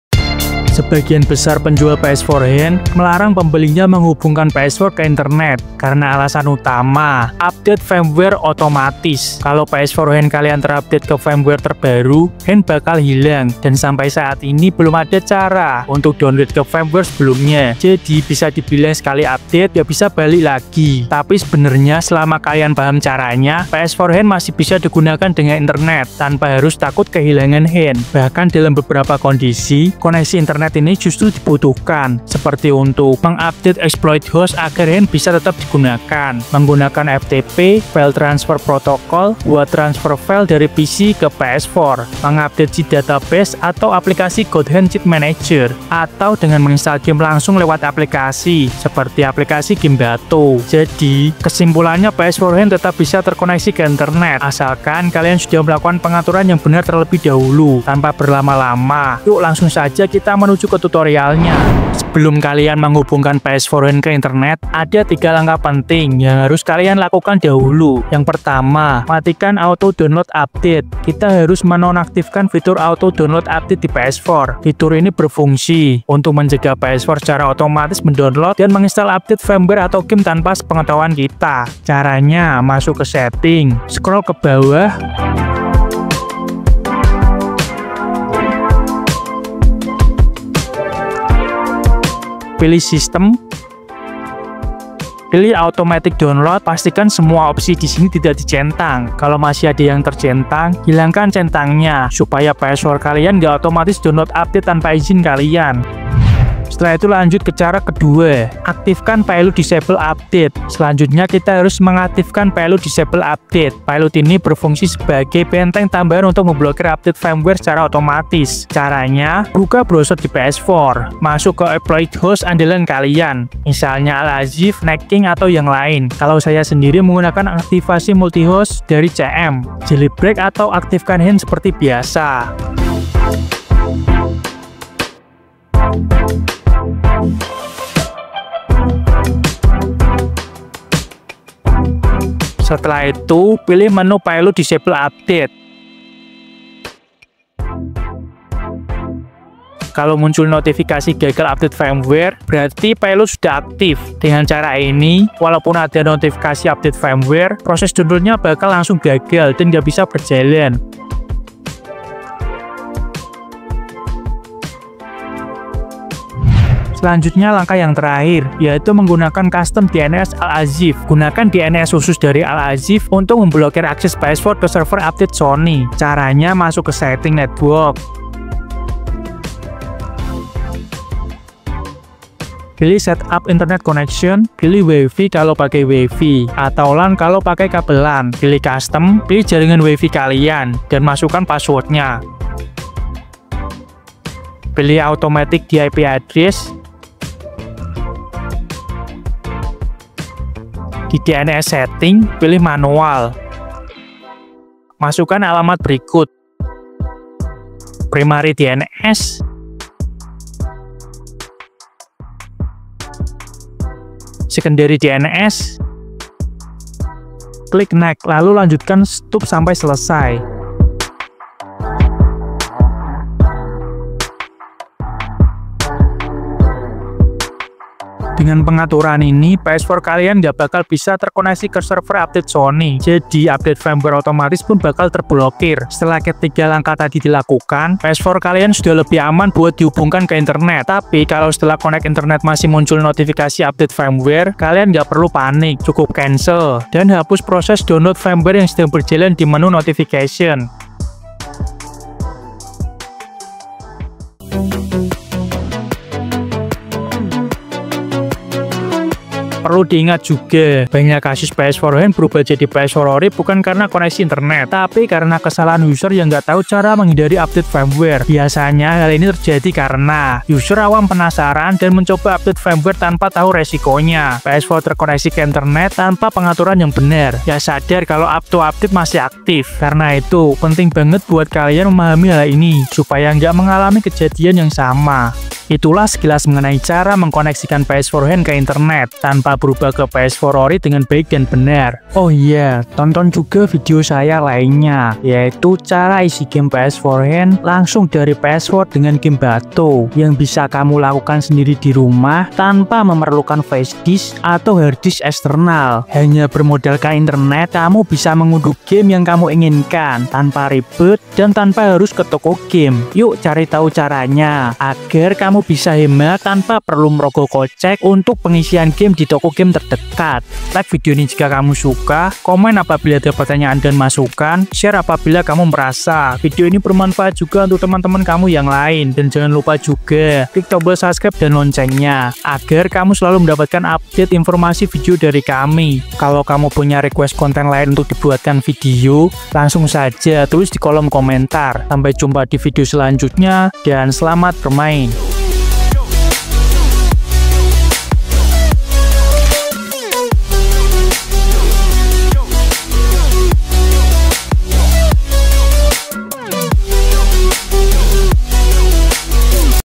Cara Mematikan Update Firmware & Menghubungkan PS4 Hen ke INTERNET!
ini dia cara mematikan update firmware ps4 hen serta menghubungkannya ke internet secara aman dan benar.

TANYA JAWAB SEPUTAR PS?!